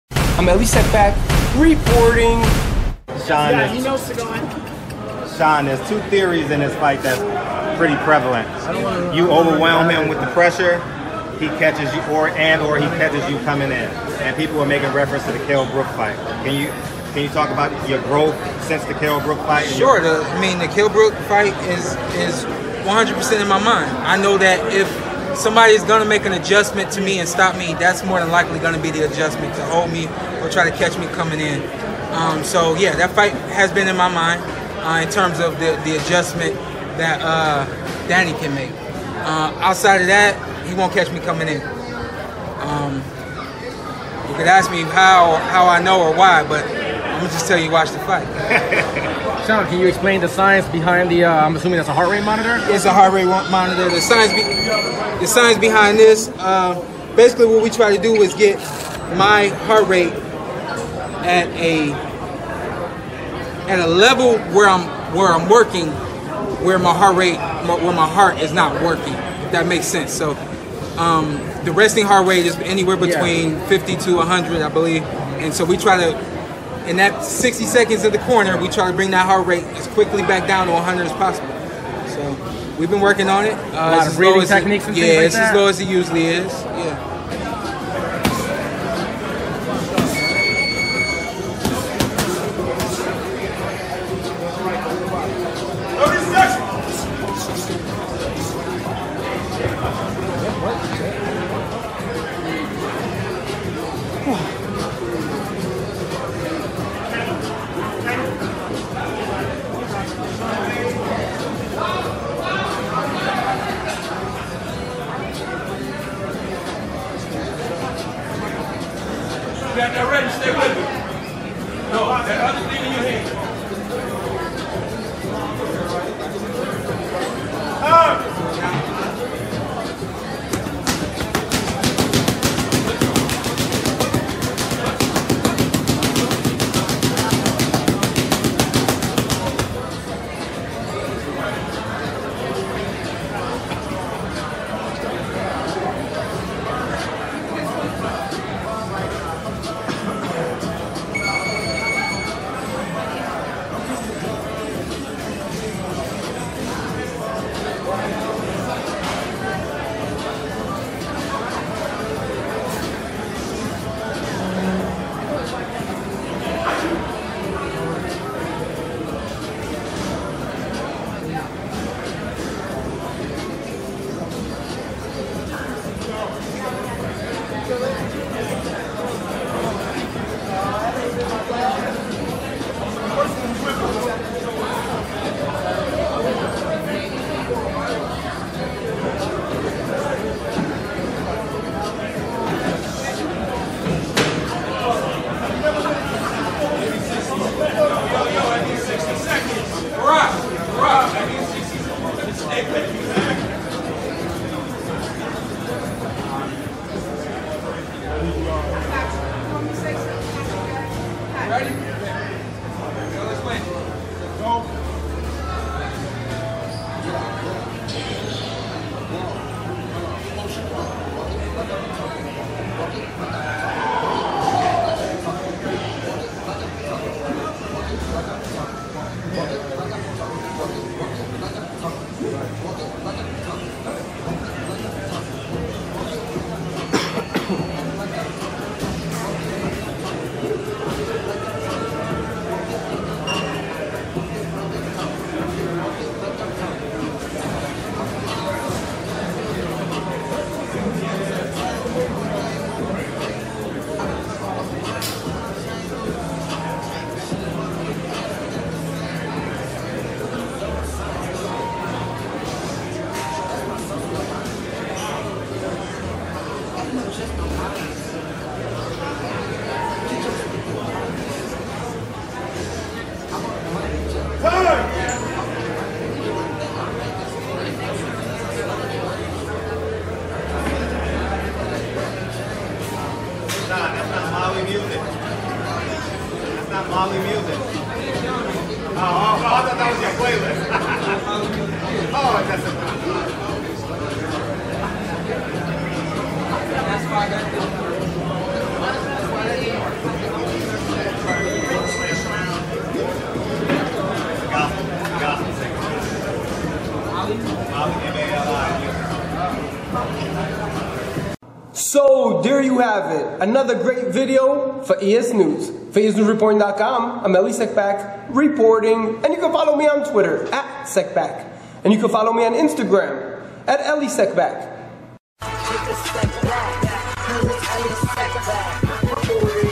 (Cutting Edge) Shawn Porter Trains With A Heart Monitor Explains Why EsNews Boxing
Shawn Porter Trains With A Heart Monitor Explains Why